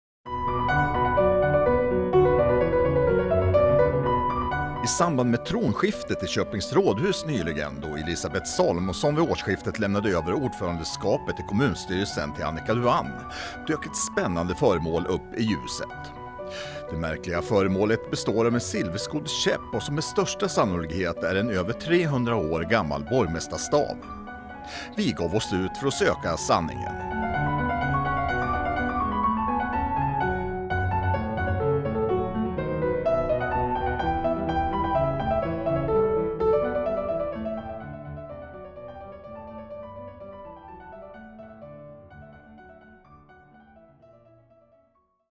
Silverkäpp på rådhuset - Västmanlands Television
I samband med tronskiftet i Köpings rådhus nyligen, då Elizabeth Salomonsson vid årsskiftet lämnade över ordförandeskapet i kommunstyrelsen till Annika Duán, dök ett spännande föremål upp i ljuset. Nämligen ett något märkligt föremål bestående av en silverskodd käpp och som med största sannolikhet är en över 300 år gammal borgmästarstav.